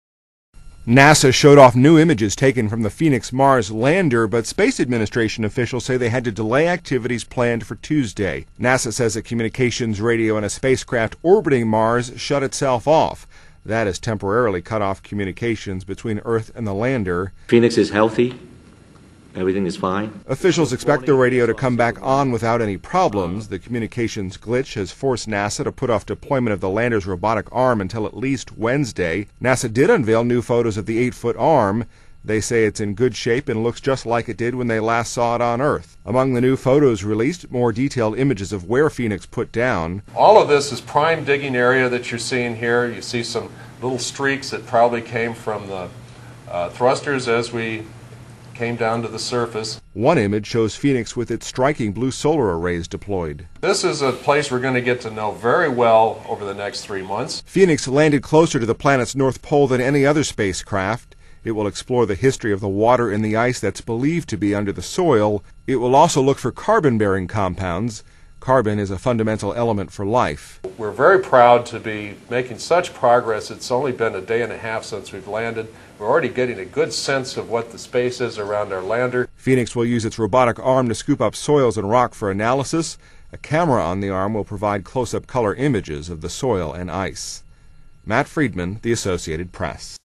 Mars Lander Activities Slightly Delayed
TechnologyMars Lander Activities Slightly DelayedMars Lander Activities Slightly DelayedThe Associated PressNASA officials say plans for the Phoenix Mars Lander's second day of activities on Mars have been delayed. (May 27)NASA showed off new images taken from the Phoenix Mars Lander, but space administration officials say they had to delay activities planned for Tuesday. NASA says a communications radio in spacecraft orbiting Mars shut itself off. That has temporarily cut off communications between earth and the lander.(SOT: FUK LI/NASA)(14:02;22) PHOENIX IS HEALTHY. EVERYTHING IS FINE. Officials expect the radio to come back on without any problems.  The communications glitch has forced NASA to put off deployment of the lander's robotic arm until, at least, Wednesday. NASA did unveil new photos of the eight-foot arm. They say it's in good shape and looks just like it did when they last saw it on earth. Among the new photos released, more detailed images of where Phoenix put down.(SOT: PETER SMITH)(14:17:40) ALL OF THIS IS PRIME DIGGING AREA. YOU SEE SOME STREAKS THAT PROBABLY CAME FROM THE THRUSTERS AS WE CAME DOWN TO THE SURFACE. One image shows Phoenix with its striking blue solar arrays deployed.(PETER SMITH)(14:18;11) THIS IS A PLACE WE'RE GOING TO GET TO KNOW VERY WELL OVER THE NEXT THREE MONTHS. Phoenix landed closer to the planet's north pole than any other spacecraft. It will explore the history of the water in the ice that's believed to be under the soil. It will also look for carbon-bearing compounds. Carbon is a fundamental element for life.(SOT: PETER SMITH/UNIVERSITY OF ARIZON)(14;20:52) WE'RE VERY PROUD TO BE MAKING SUCH GOOD PROGRESS. IT'S ONLY BEEN A DAY AND A HALF SINCE WE'VE LANDED. 59 WE'RE ALREADY GETTING A GOOD SENSE OF WHAT THE SPACE IS AROUND OUR LANDER.Phoenix will use it's robotic arm to scoop up soils and rock for analysis. A camera on the arm will provide close up color images of the soil and ice.     ___ ___, The Associated Press.